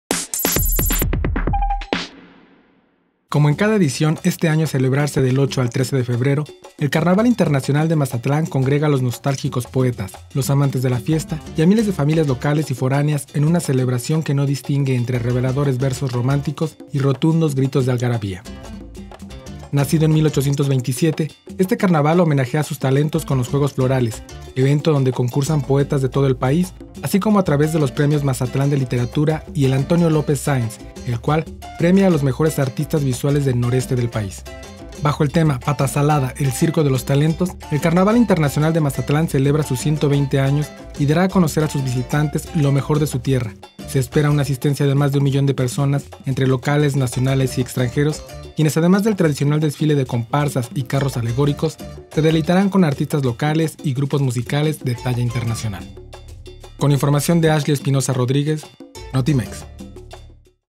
Carnaval de Mazatlán, el lugar donde covergen la cultura y la fiesta
Bajo el tema "Patasalada, el circo de los talentos", el Carnaval Internacional de Mazatlán celebrará sus 120 años.